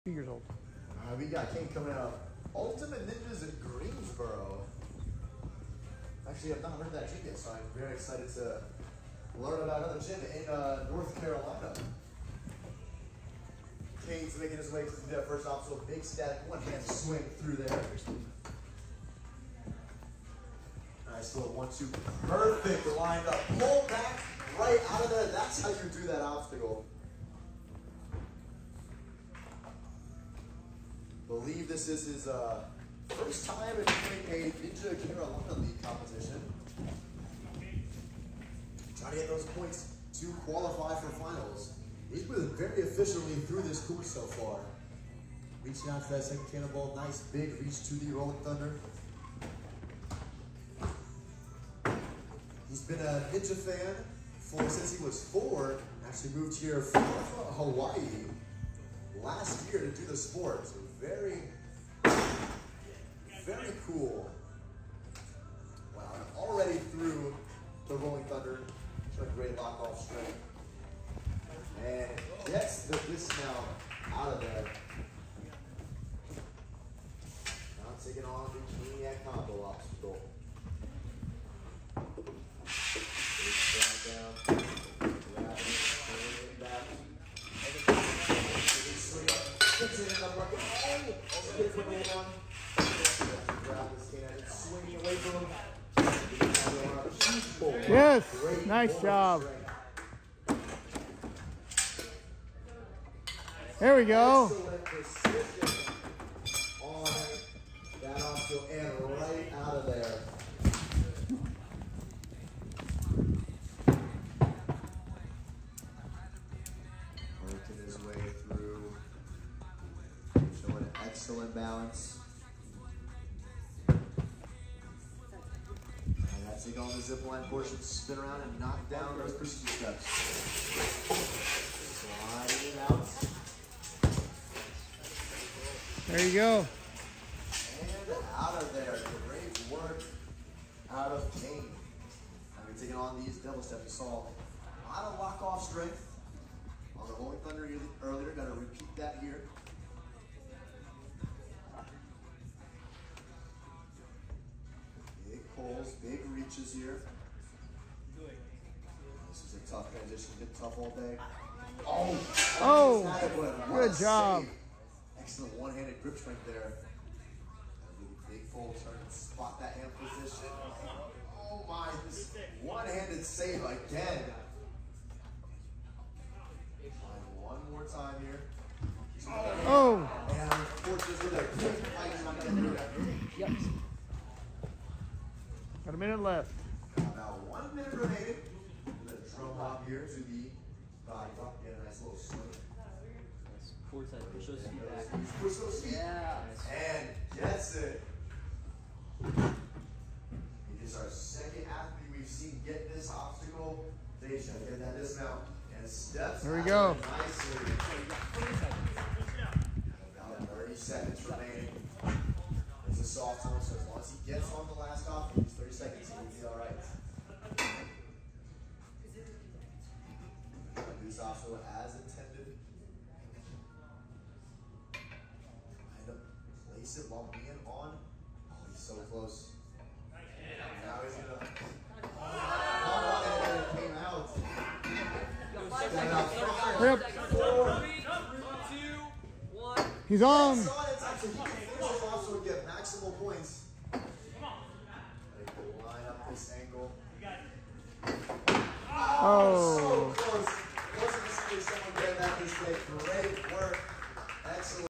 My 2nd Place Run at Rock Solid!
MAN I wish I grabbed onto that Special Delivery. Tied off points in my first NCL competition, but lost on time. One extra beaten obstacle would have done it for me. Still a great time though, can't wait for the next one!